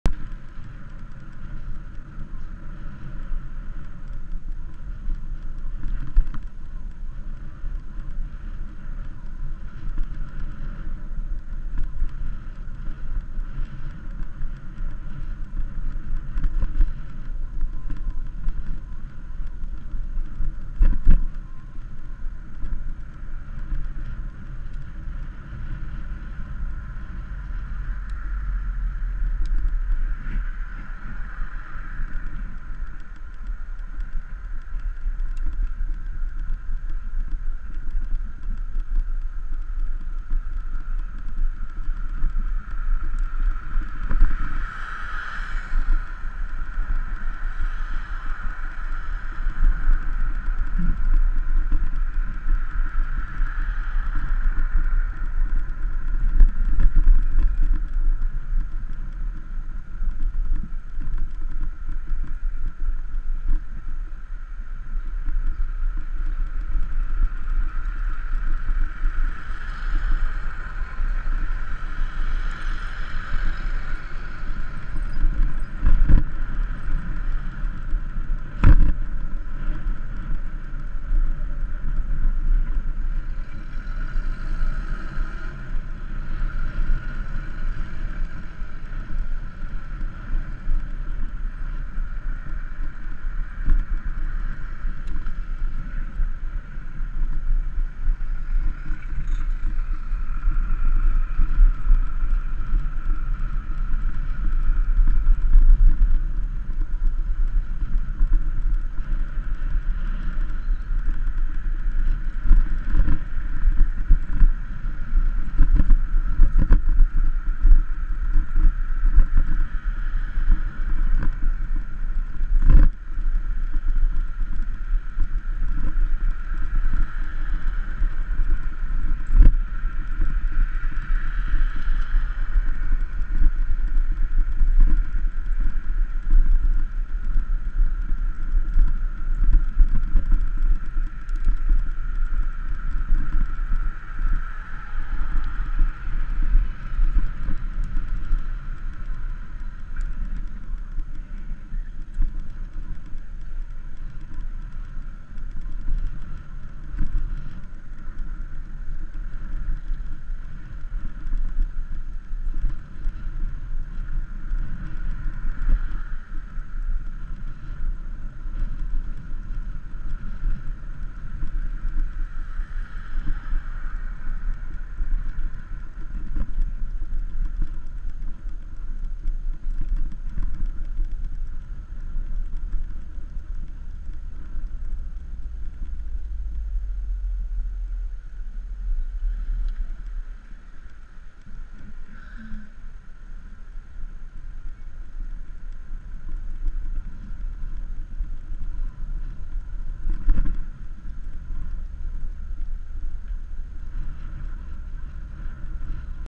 Parte del trayecto al trabajo en bicicleta
Un corto trayecto camino a mi trabajo, se puede apreciar como entro a la rotonda y como salgo de ella, ademas, el momento de señalizar con los brazos al momento de girar y de como los conductores respetan, un aplauso para ellos, Pedalea por la calle sin miedo, pero con mucha precaución, Arica es una ciudad amable aun, aprovechemos :)